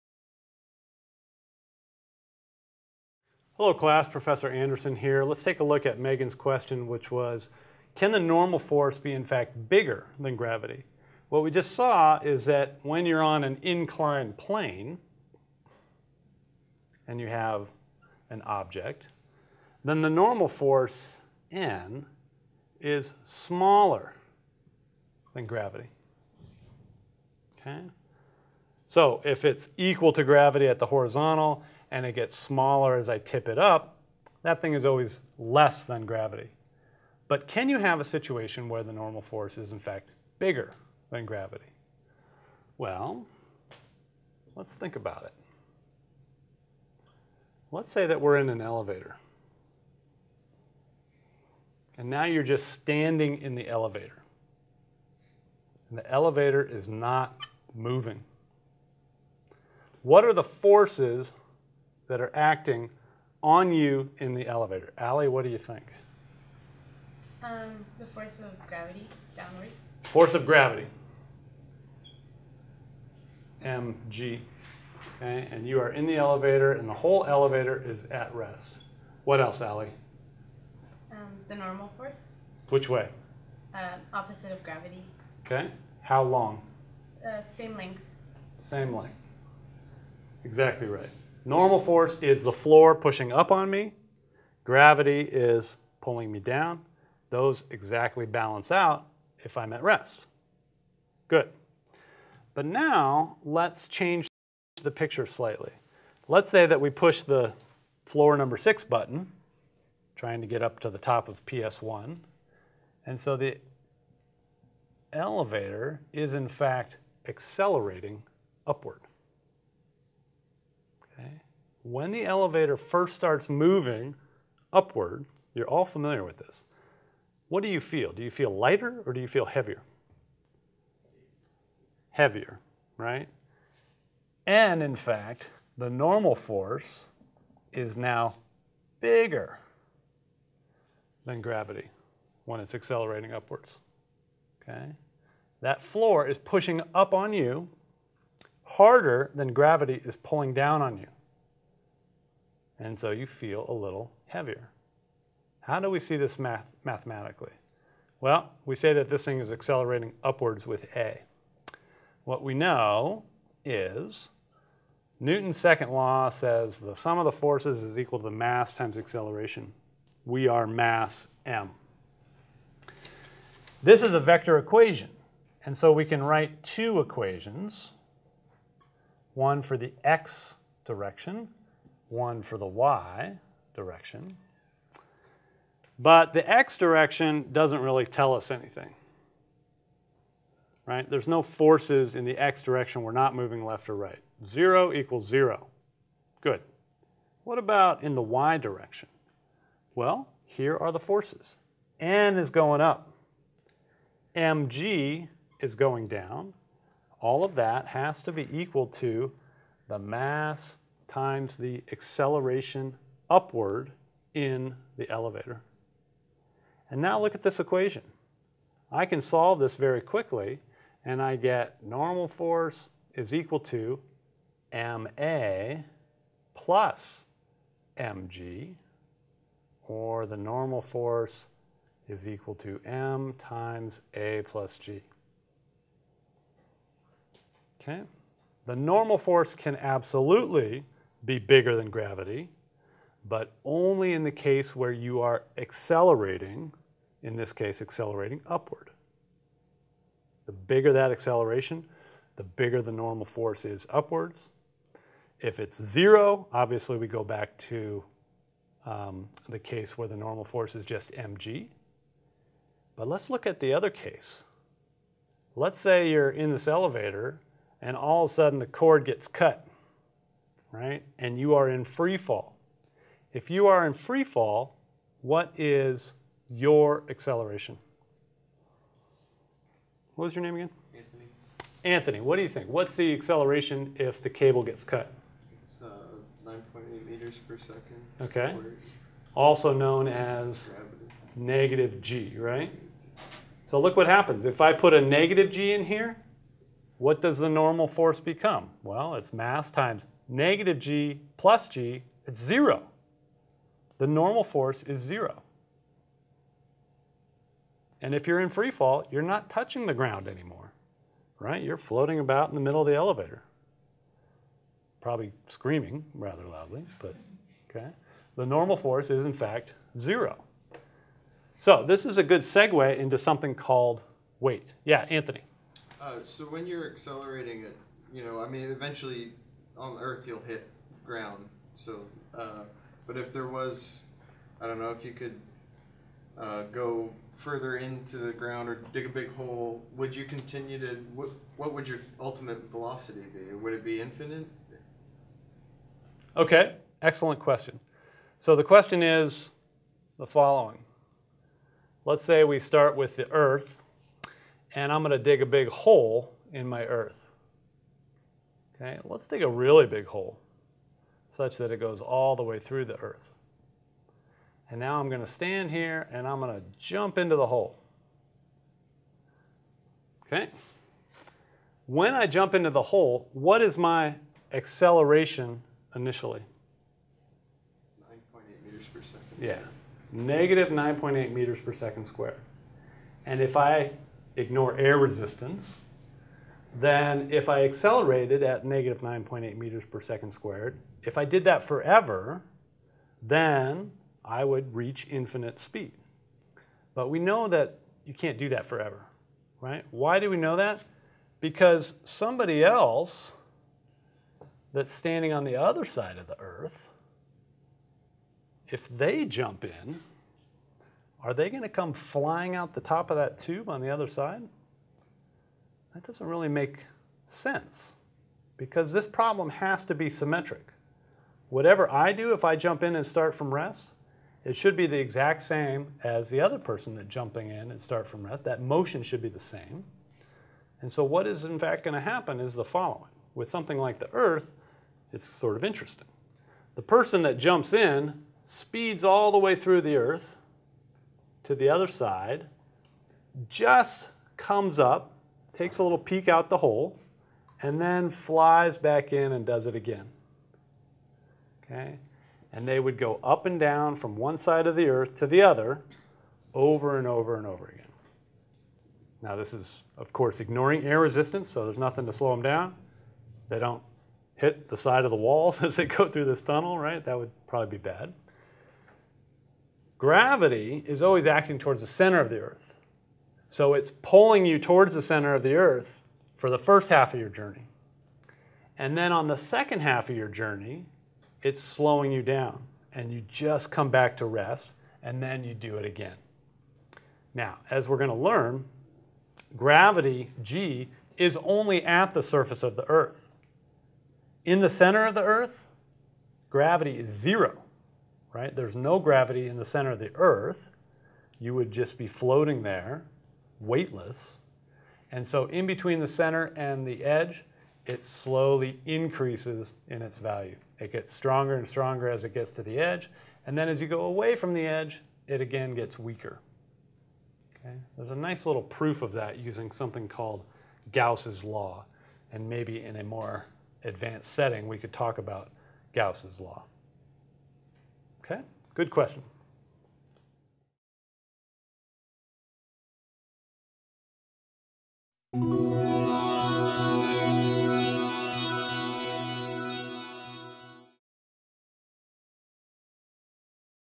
Weight in an elevator
Can the normal force be bigger than your gravitational weight?